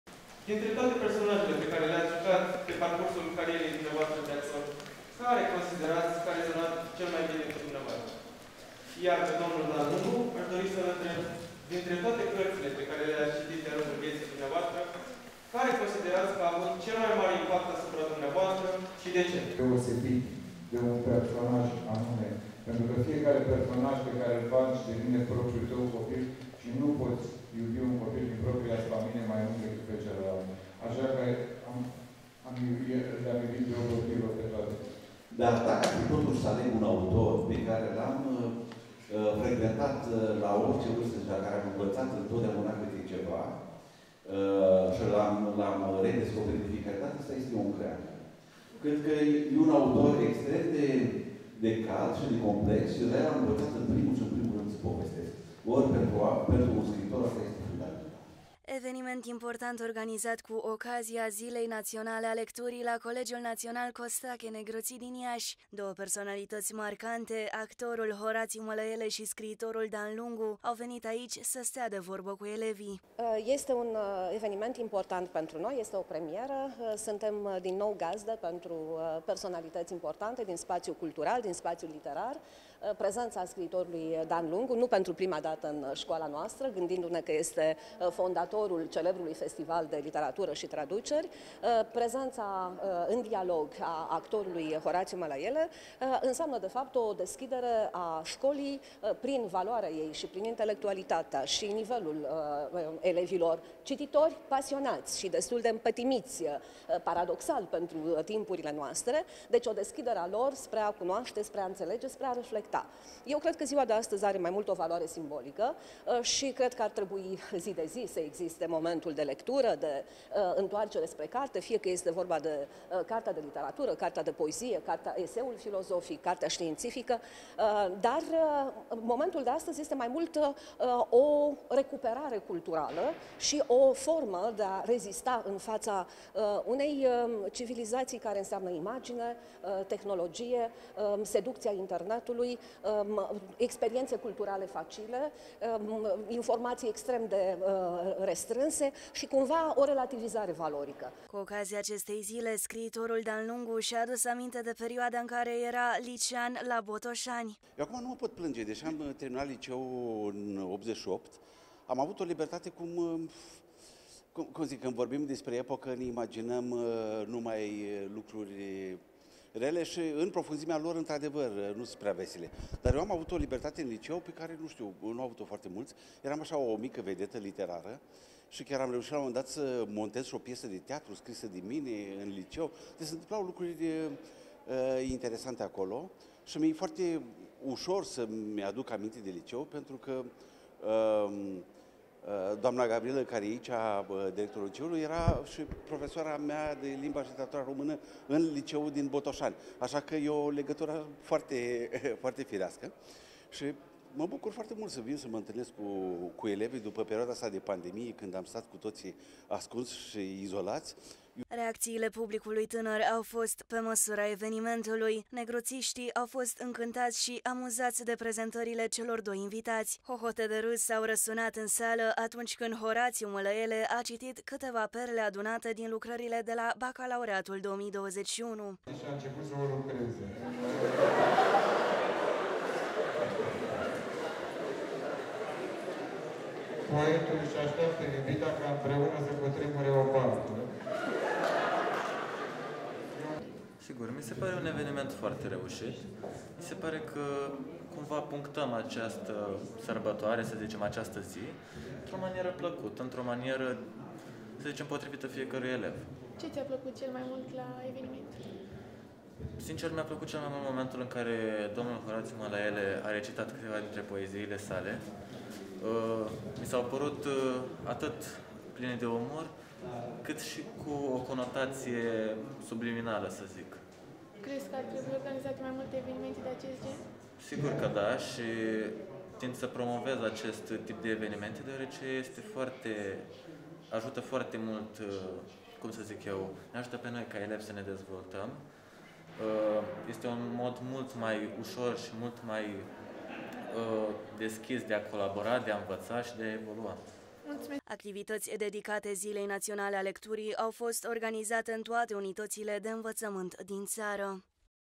Personalitati printre liceeni, de Ziua Lecturii
Sute de elevi de la Colegiul National Costache Negruzzi au marcat Ziua Nationala a Lecturii prin interactiune. Cunoscutul actor Horatiu Malaele si scriitorul iesean  Dan Lungu au fost invitatii evenimentului. Adolescentii au aflat diferite curiozitati si s-au distrat copios la glumele facute de cei doi.